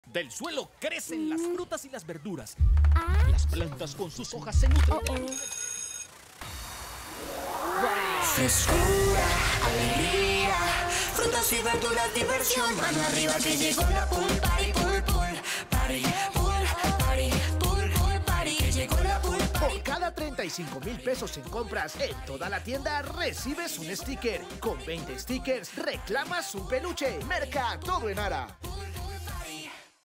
Únete a la Pulparty
¡Llegó el gran día a tiendas ara para conocer a los artistas más divertidos que llenarán tus días de música, sabor y alegría!